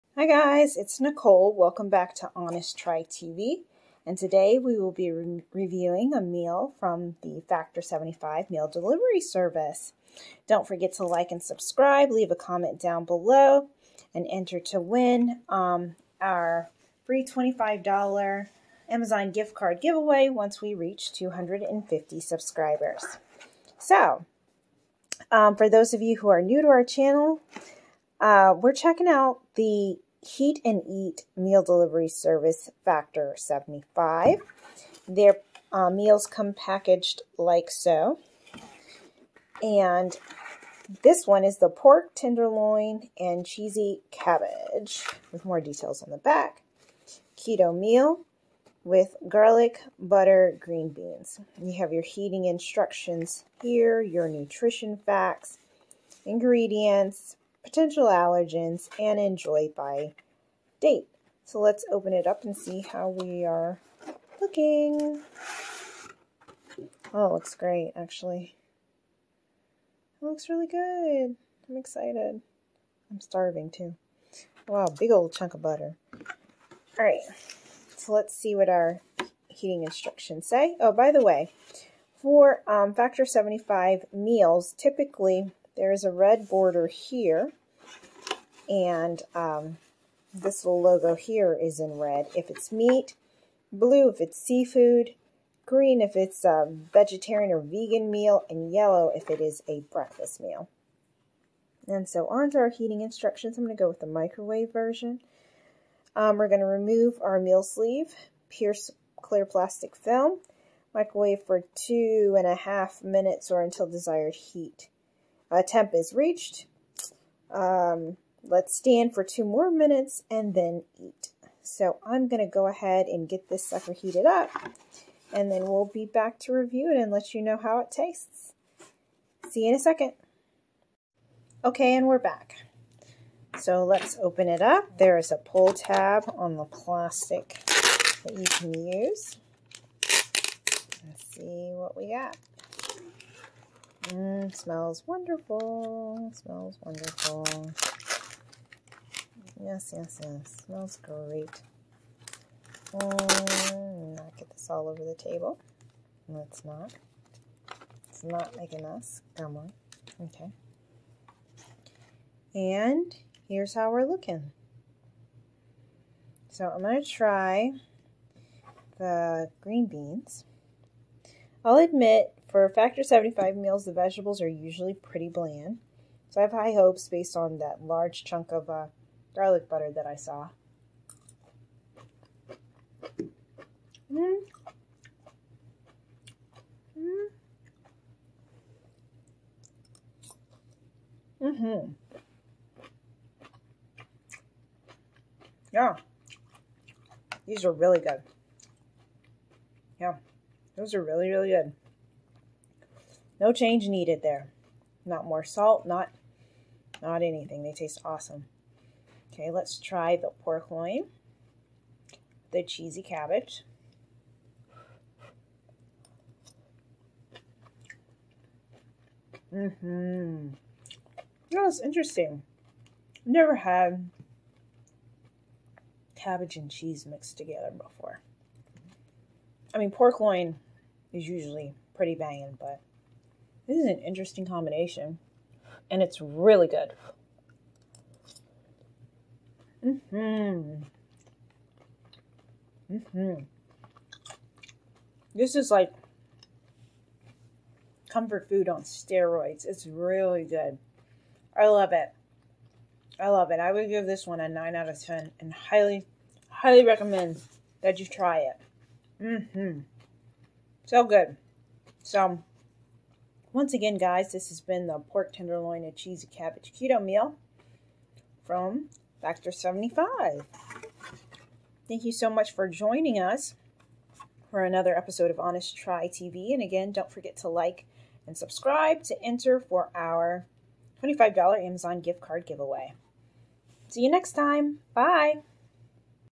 Factor 75 Review - Pork Tenderloin & Cheesy Cabbagewith Garlic Butter Green Beans | Noobs at Work
We review the Pork Tenderloin & Cheesy Cabbage with Garlic Butter Green Beans meal from Factor 75. Watch us noobs at work, giving it an honest try on making, rating and reviewing this dish.